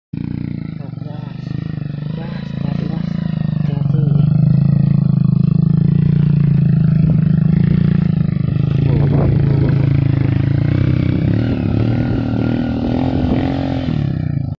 APA YANG ANDA PIKIRKAN!! ketika asyik mancing Nemu orang heroick
apa yang anda pikirkan. ketika asyik mancing menemukan orang heroick di dunia#shorts

Assalamualaikum... Manteman semuanya🙏🙏🙏
Mukbang.motor grandong,